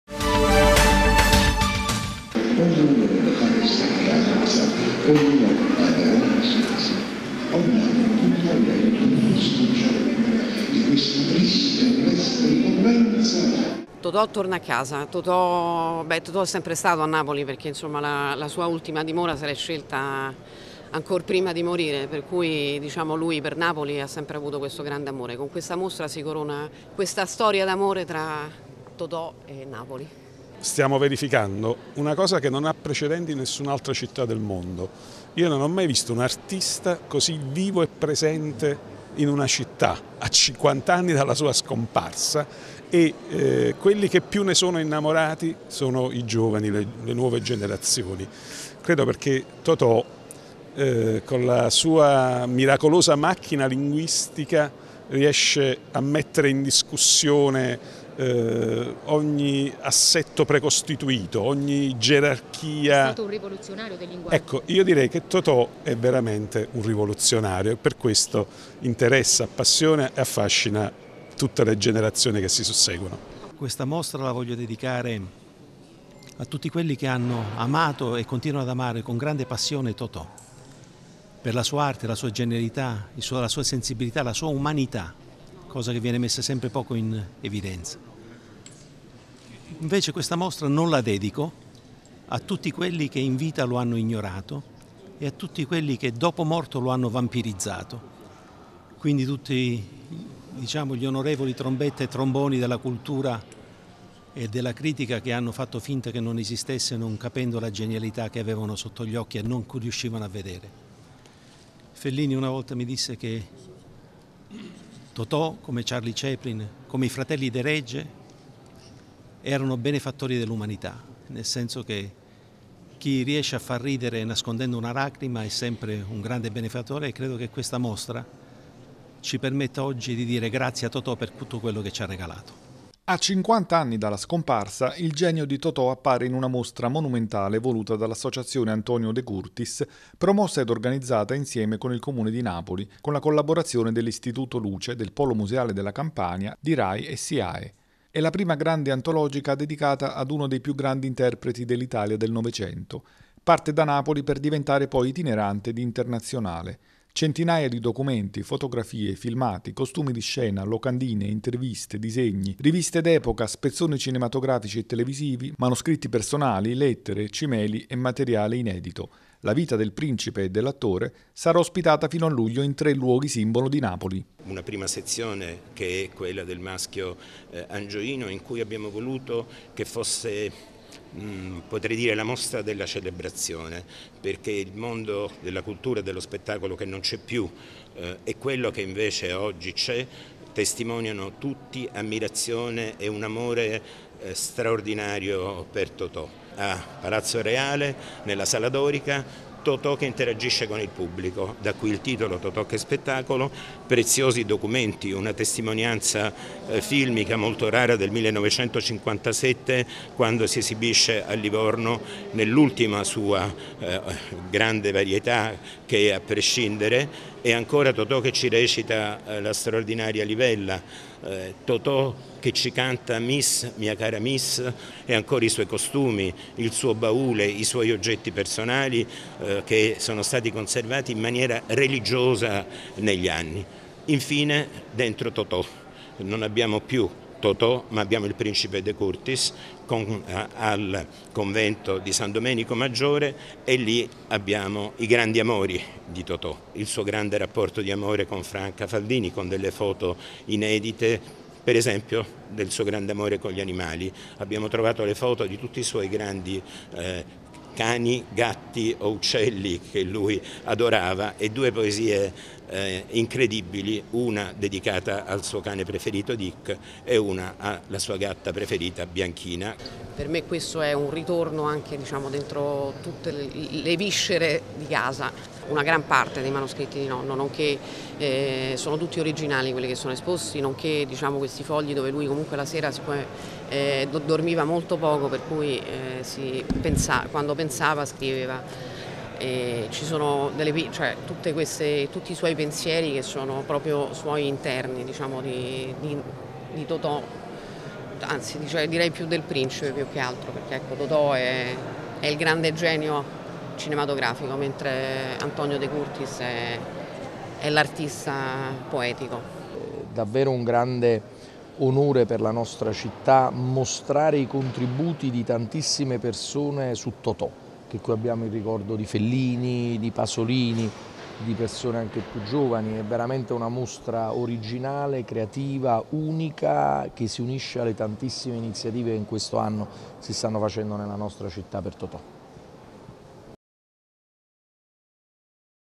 Totò Genio: il Principe e l'attore
Maschio Angioino, Palazzo Reale e San Domenico Maggiore: tre sedi per una mostra che pare da Napoli per diventare poi internazionale: a 50 anni dalla scomparsa, il genio di Totò offerto al pubblico attraverso centinaia di documenti che raccontano la sua vita pubblica e privata.